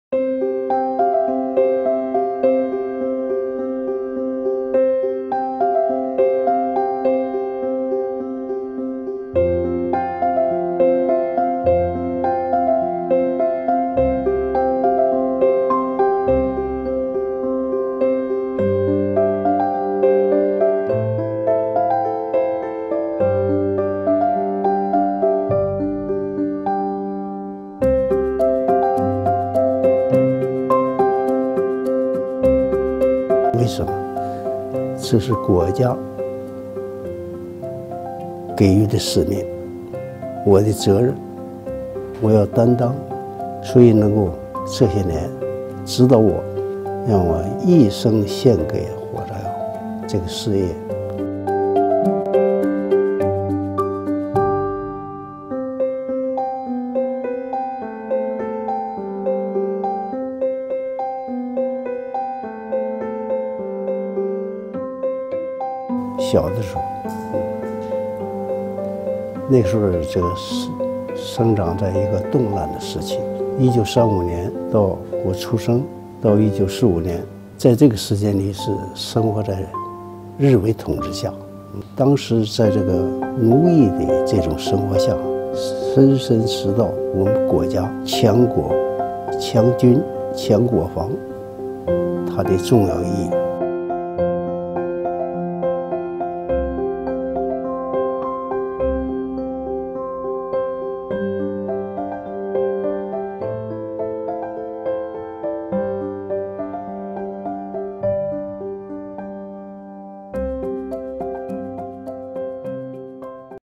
China's "king of gunpowder" wins national science award for 4th time
Meet China's leading researcher on explosives, 83-year-old Wang Zeshan! He dedicated his life to China's national defense.